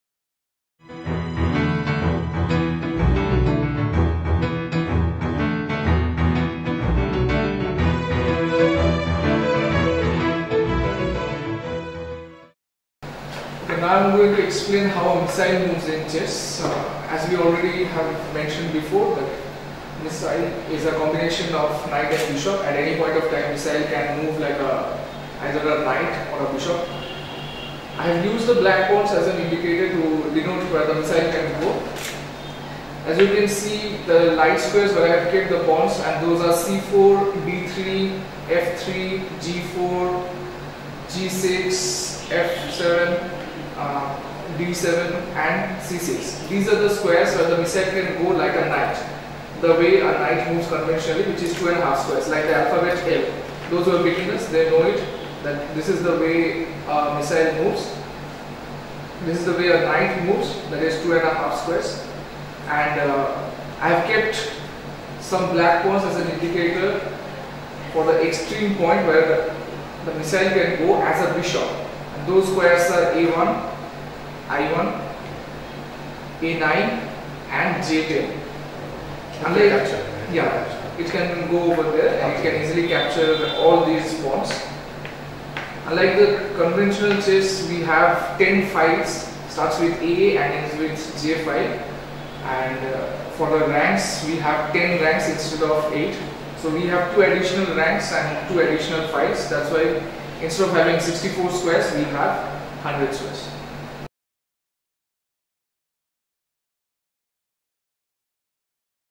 Missile Movements & Coordinates
In this video the movement of 'Missile' has been discussed. Addition of 2 new files & ranks have also been explained.